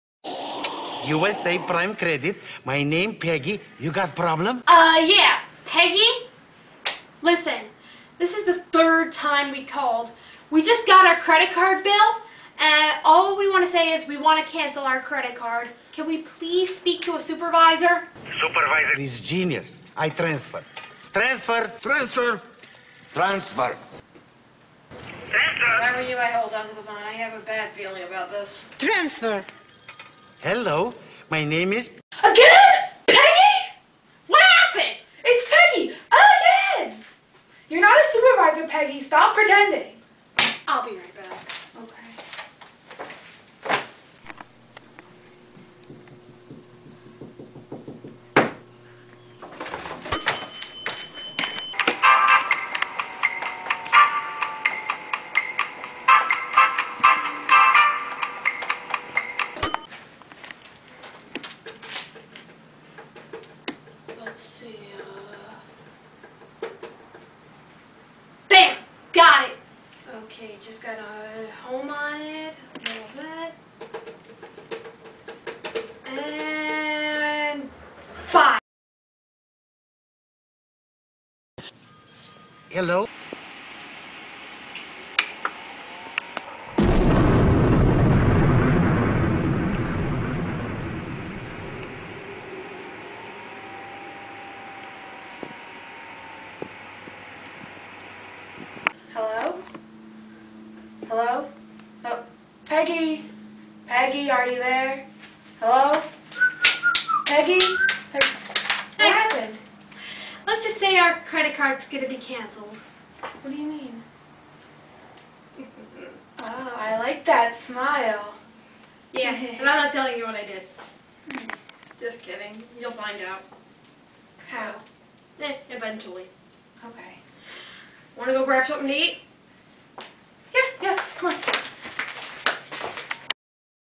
Bye Peggy
Jayne And Lucy Call USA Prime Credit to cancel their card